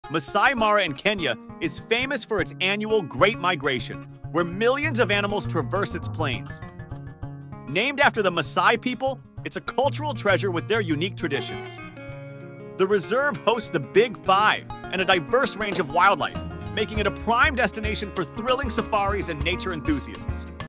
"Discovering Masai Mara: Kenya's Wildlife Wonderland | Safari Adventure 2023"
Embark on an epic safari through the iconic Masai Mara in Kenya, a wildlife enthusiast's dream come true. This travel vlog unveils the raw beauty of the savannah, showcasing breathtaking animal encounters and the timeless culture of the Maasai people. From the Great Migration to the Big Five, get ready for an unforgettable African adventure. MasaiMara SafariAdventure KenyaWildlife"